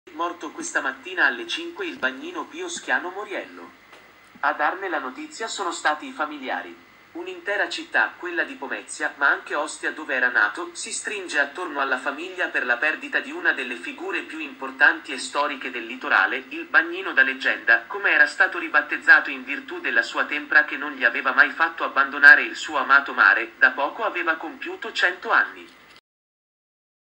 TORVAIANICA IN LUTTO Si è Spento Nella Notte Pio Schiano Moriello IL Bagnino Centenario.
Torvaianica, è morto Pio il bagnino centenario: è rimasto in spiaggia fino all’ultimo giorno.